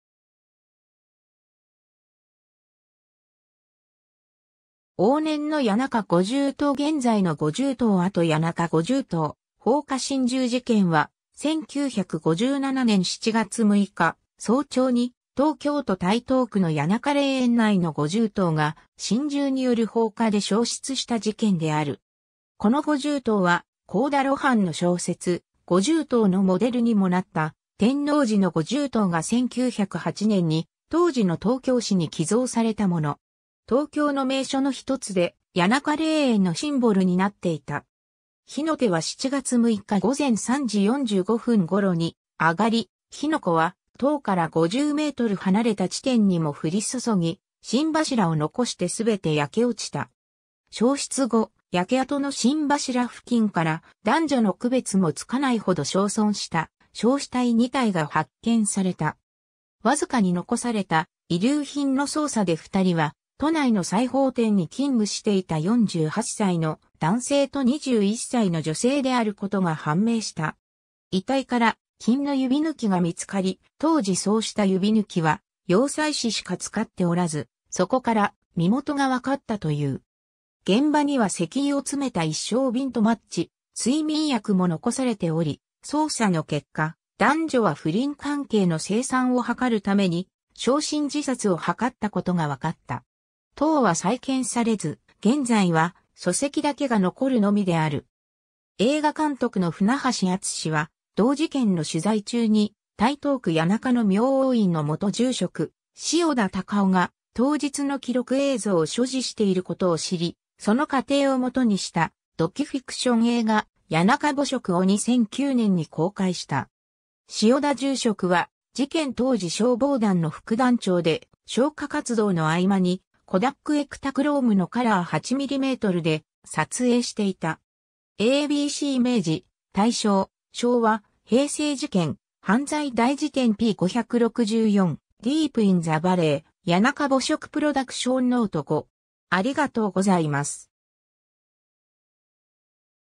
谷中五重塔放火心中事件
往年の谷中五重塔 現在の五重塔跡 谷中五重塔放火心中事件（やなかごじゅうのとうほうかしんじゅうじけん）は、1957年（昭和32年）7月6日早朝に東京都台東区の谷中霊園内の五重塔が、心中による放火で焼失した事件である。
 この五重塔は、幸田露伴の小説『五重塔』のモデルにもなった天王寺の五重塔が1908年（明治41年）に当時の東京市に寄贈されたもの。
東京の名所のひとつで、谷中霊園のシンボルになっていた。
 火の手は7月6日午前3時45分ごろに上がり、火の粉は塔から50m離れた地点にも降り注ぎ、心柱を残してすべて焼け落ちた。
焼失後、焼け跡の心柱付近から男女の区別も付かないほど焼損した焼死体2体が発見された。
わずかに残された遺留品の捜査で2人は都内の裁縫店に勤務していた48歳の男性と21歳の女性であることが判明した。
遺体から金の指抜きが見つかり、当時そうした指抜きは洋裁師しか使っておらず、そこから身元がわかったという。
 現場には石油を詰めた一升ビンとマッチ、睡眠薬も残されており、捜査の結果、男女は不倫関係の清算を図るために焼身自殺を図ったことがわかった。
 塔は再建されず、現在は礎石だけが残るのみである（2007年に塔の再建計画が報道されている）。
 映画監督の舩橋淳は同事件の取材中に台東区谷中の明王院の元住職、塩田隆雄が当日の記録映像を所持していることを知り、その過程をもとにしたドキュフィクション映画『谷中暮色』を2009年に公開した。
塩田住職は、事件当時消防団の副団長で、消火活動の合間に、コダックエクタクロームのカラー8mmで撮影していた。
 ^ a b c 明治・大正・昭和・平成 事件・犯罪大事典 p.564 ^ Deep in the valley 谷中暮色 PRODUCTION NOTE 5